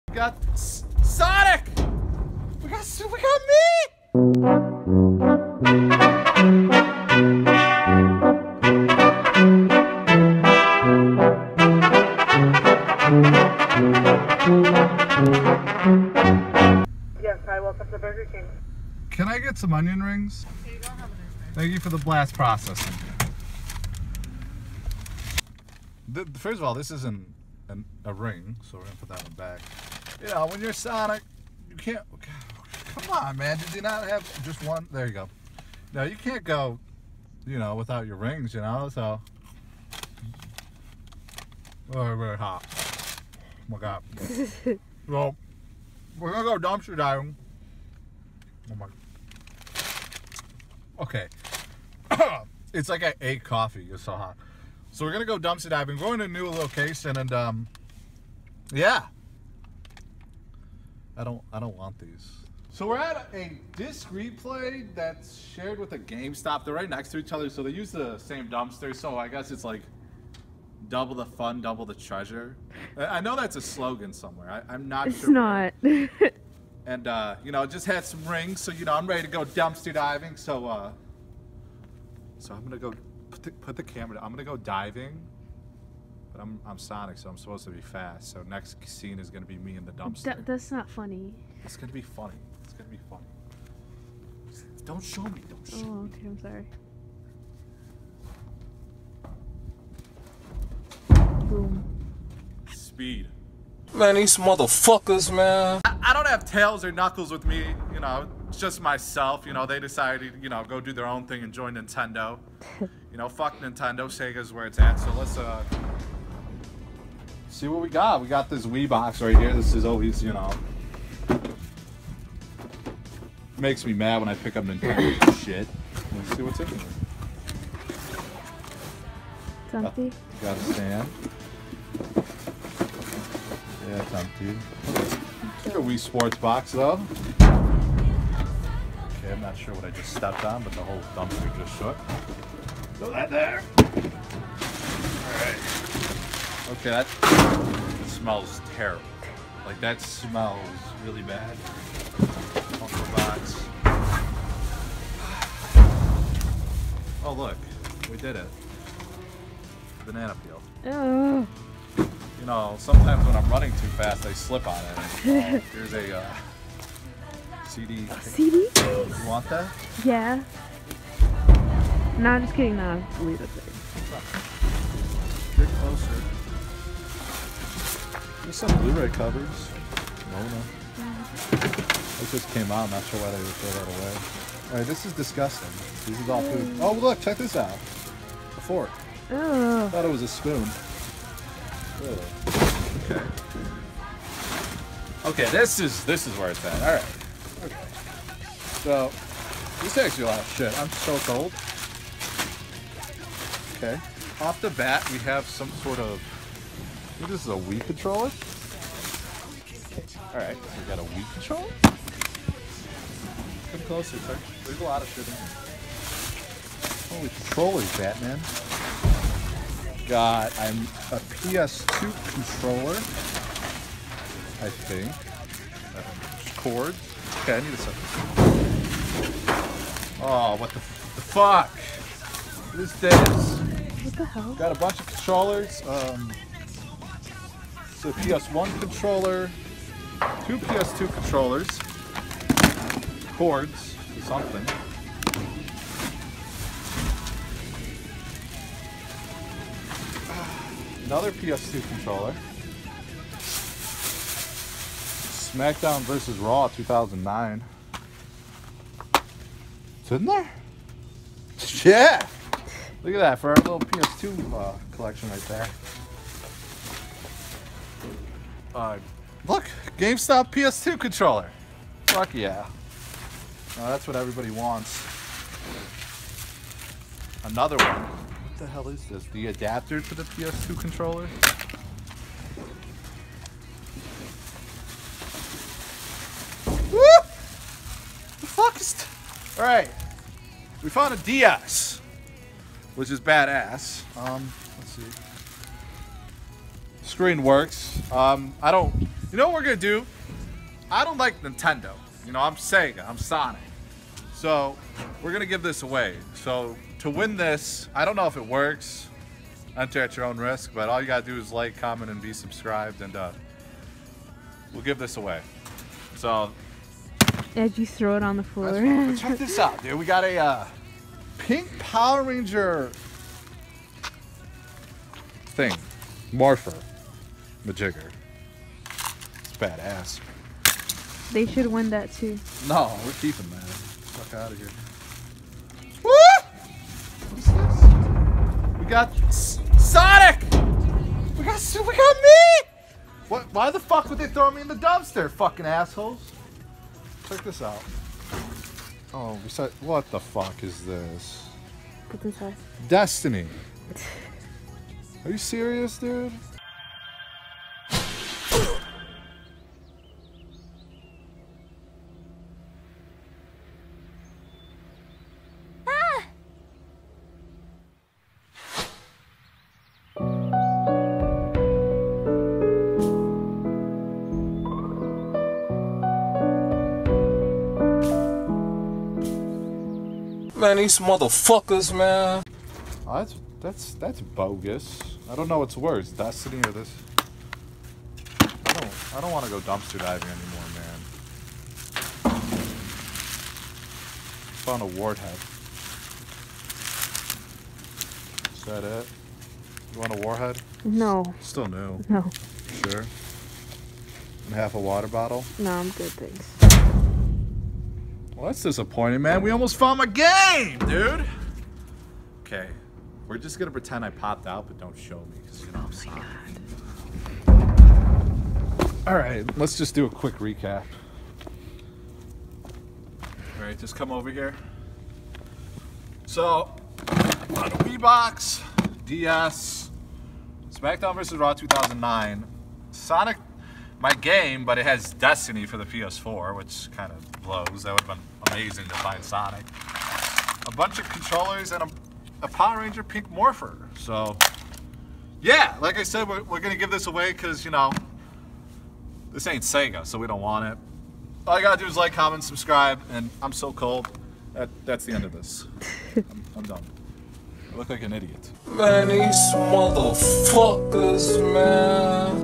Sonic The Hedgehog Goes Dumpster Diving At GAMESTOP!!!
Who is better at Dumpster Diving than sonic the hedgehog? Noone duh!